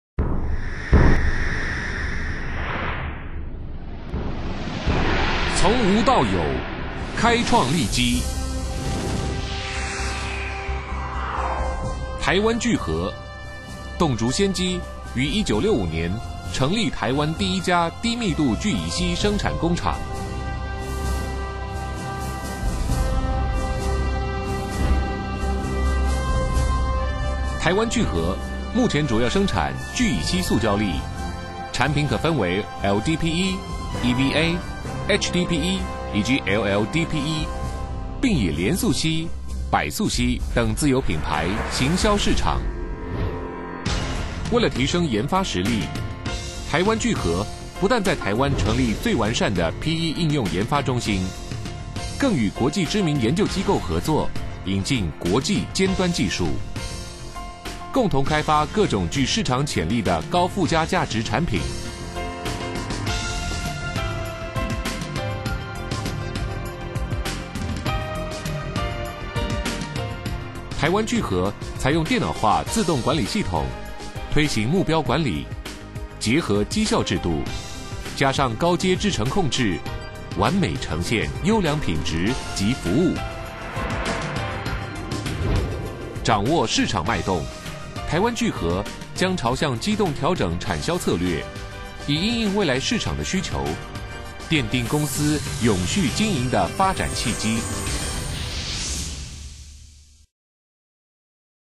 台湾聚合化学品股份有限公司 - 介绍影片_2012
台湾聚合化学品股份有限公司设立于民国五十四年五月二十六日。

主要业务为设计、开发、制造与销售聚乙烯塑料粒，工厂设于高雄市仁武区。本公司生产之聚乙烯分为低密度聚乙烯 (LDPE) 、乙烯醋酸乙烯酯 (EVA) 、高密度聚乙烯 (HDPE) 、线型低密度聚乙烯 (LLDPE) 四种。聚乙烯塑料粒经过下游厂商加工后，制成日常生活必需之各种塑料用品。

本公司产品之主要原料是乙烯，由中油公司第三、四、五轻油裂解场供应，供应量约达本公司总产能所需的三分之二。为充份运用有限之乙烯配量，本公司将尽量生产利润较高之 EVA 及 LDPE，并亦将积极开拓 LLDPE 市场。